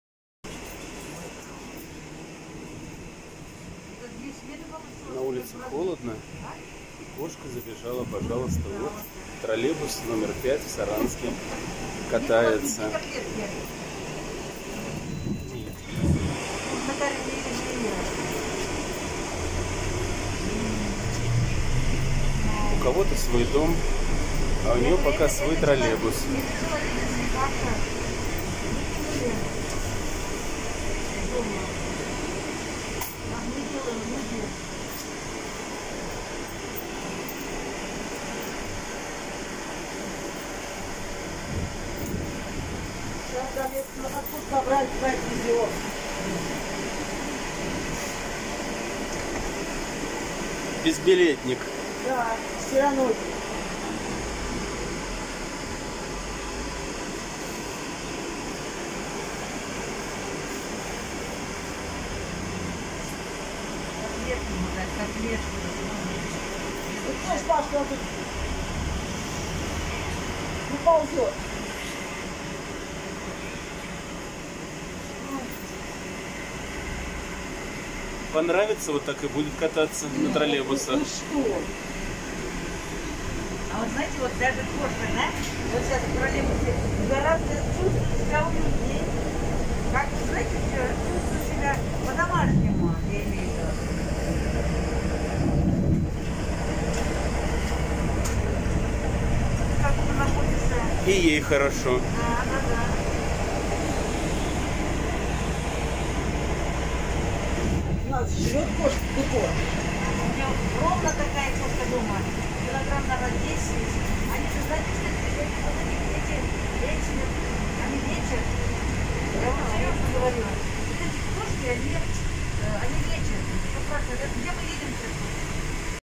Четвероногий безбилетник!
Милый котик - безбилетный пассажир троллейбуса N 5 в Саранске. За окном - 15 градусов. В троллейбусе стало уютно, по-домашнему!